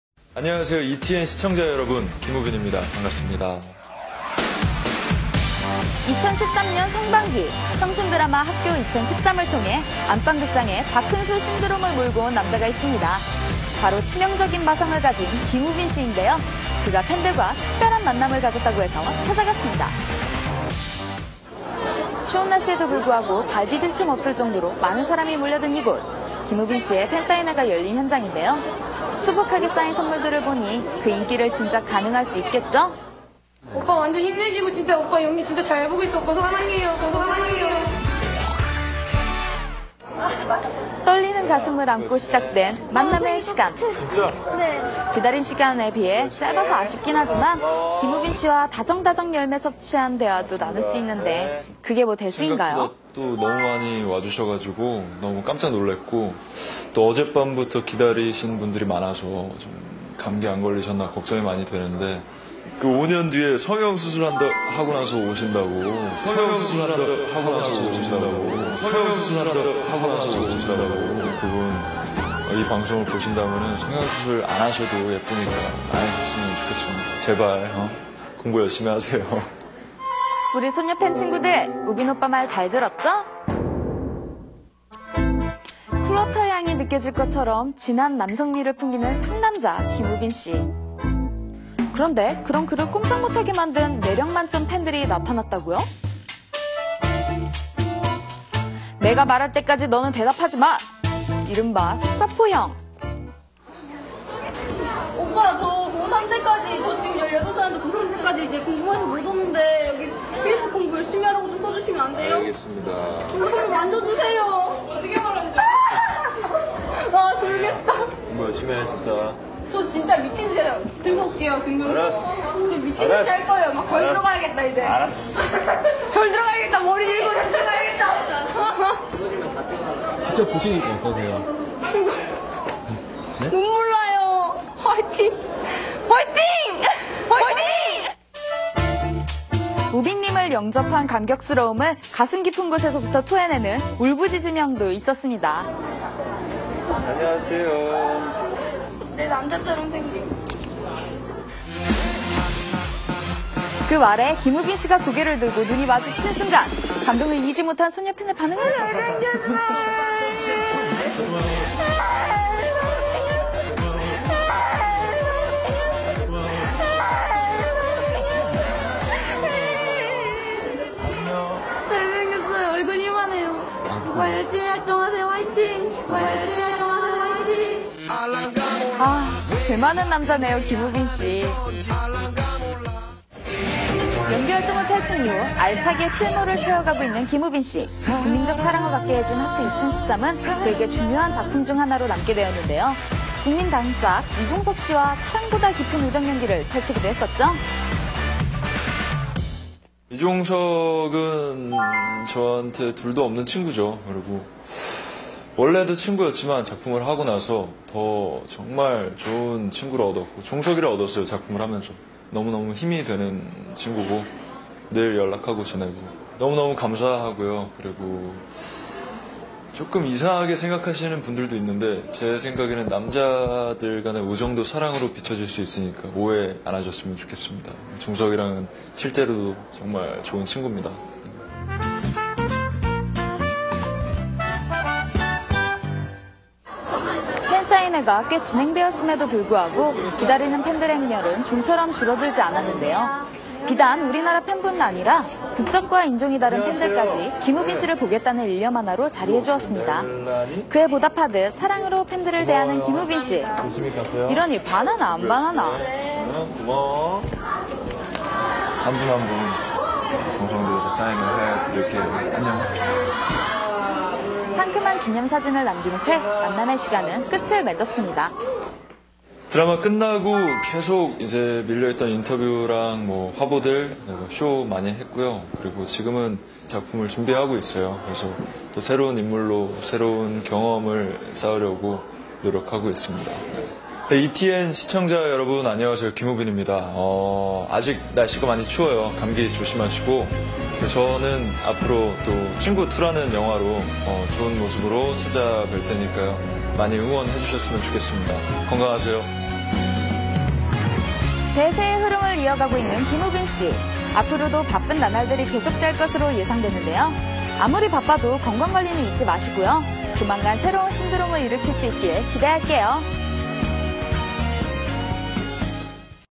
Woobin ETN interview
"To me, Lee Jongsuk is a friend like no other. We're already friends originally, but through doing works (together) I realize (that he's) a good friend and I gained Jongsuk (as friend). He's a friend who makes me strong, always calling (me) and (asked me) how am I doing, I'm very very grateful. Of course there are some people who think weirdly about this. The way I think about it, the friendship between guys can also (make that person) shines like when (that guy) is in love, so I would like for people to not misunderstand about it. Jongsuk is a really good friend of mine."

Translated by deenakahara@tumblr